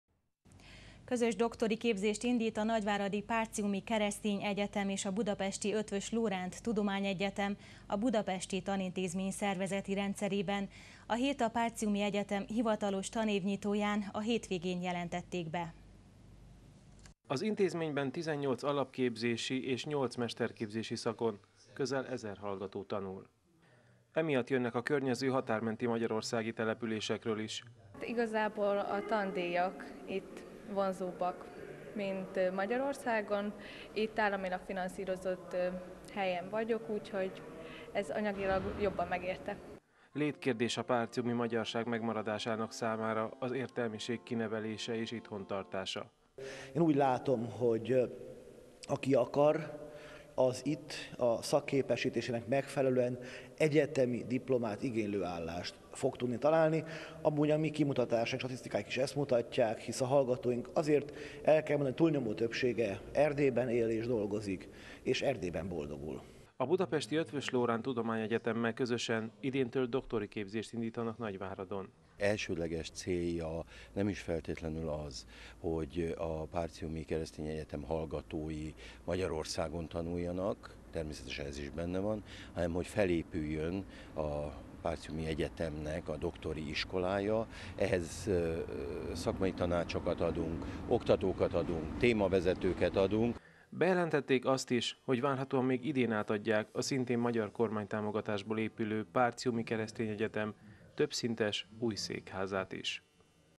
Doktori képzést indít a PKE és az ELTE – Erdélyi Magyar Televízió
Közös doktori képzést indít a nagyváradi Partiumi és a budapesti Eötvös Loránd Tudományegyetem, a budapesti tanintézmény szervezeti rendszerében. Ezt a Partiumi Egyetem hivatalos tanévnyitóján, hétvégén jelentették be.